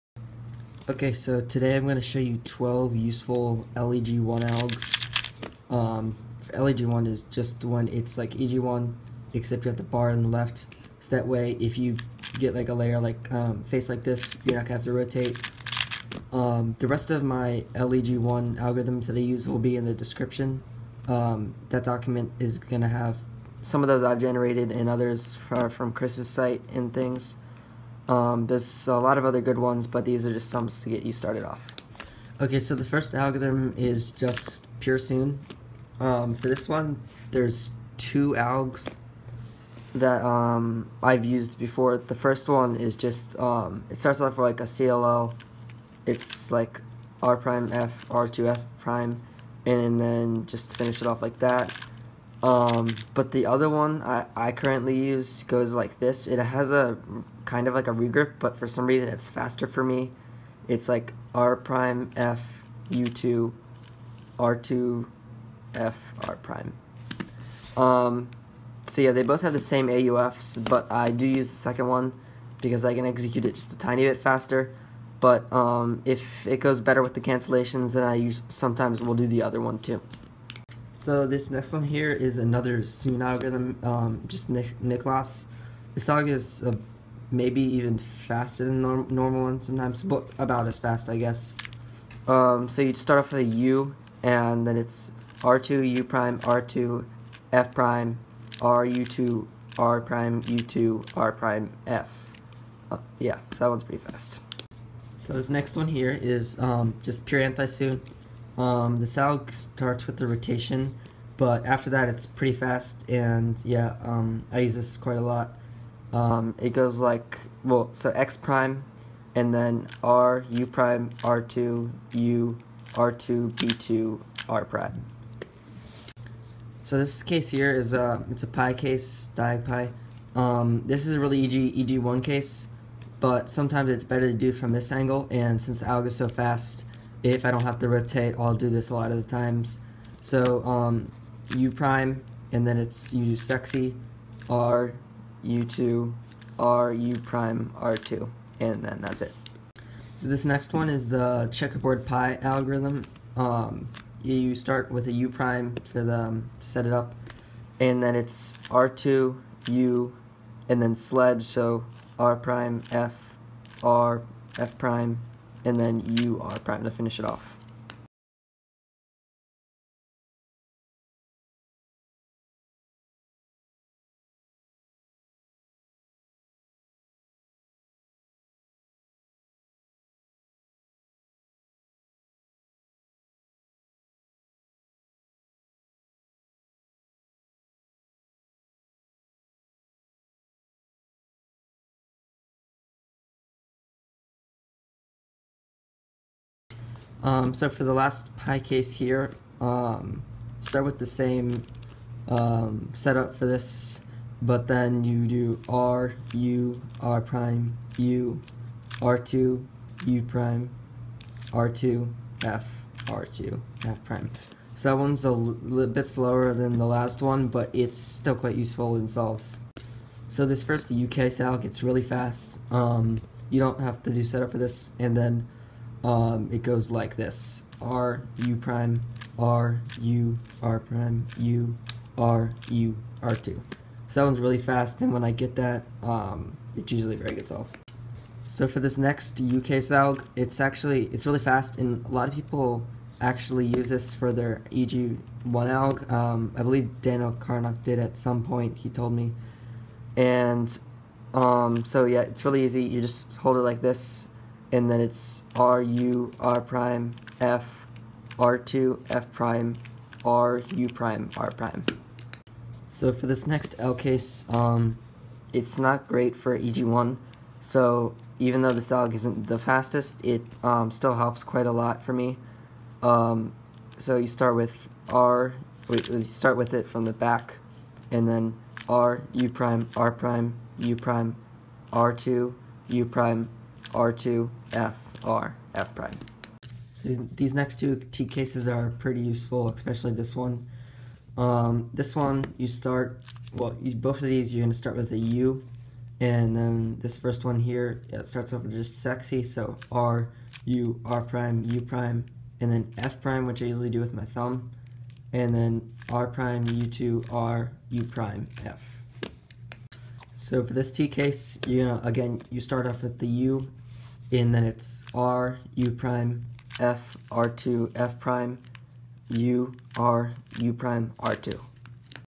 12 Useful LEG-1 Algs
Sorry number 6 is missing sound, check alg down below
Algs in the Video
1. (U2) R' F R2 F' R2 U2 R
(U2) R' F U2 R2 F R'
2. (U) R2 U' R2 F' R U2 R' U2 R' F
3. (U2) x' R U' R2 U R2 B2 R'
4. (U') R U R' U' R U2 R U' R2
5. (U') R2 U R' F R F' U R'
6. (U) R U' R' F R F' R2 U2 R
7. (U) R U R' U R2 U' R2 F R2 F'
8. R U' R U R' U R U R2
9. (U) R U R' F R2 F' R U' R'
10. (U2) R U' R' U' R2 U' R2 F R F'
11. (U) R U R' U' F' R' U2 R U' F
12. (U) R U' F R2 F' U R U' R2